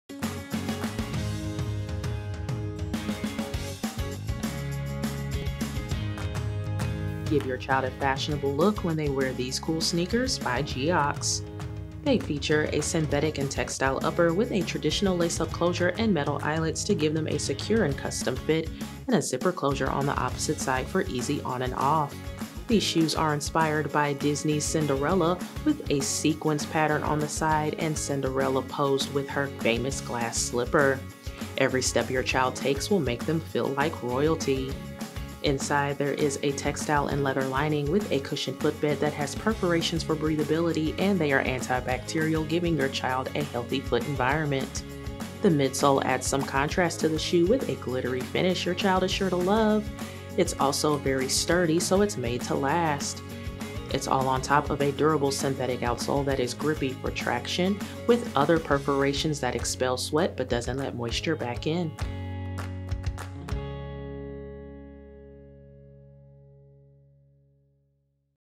Geox Kids Disney Princess Kalispera 48 (Toddler/Little Kid/Big Kid) SKU: 9611097
Please click here to view a Geox&#174; Kids footwear size guide.
Keep them in ultimate comfort and style with a classic high-top silhouette, it's the Geox&reg; Kids  Kalispera 48 sneaker.
Leather and synthetic upper with laces and side zip entry.
Leather and textile lining and insole.
Princess print sequins on sides.
Durable synthetic outsole.
Imported.
Geox&#8482; patented breathable shoe for active kids features a unique microporous membrane that absorbs and expels sweat while keeping water out.

Product measurements were taken using size 24 (US 8 Toddler), width M. Please note that measurements may vary by size.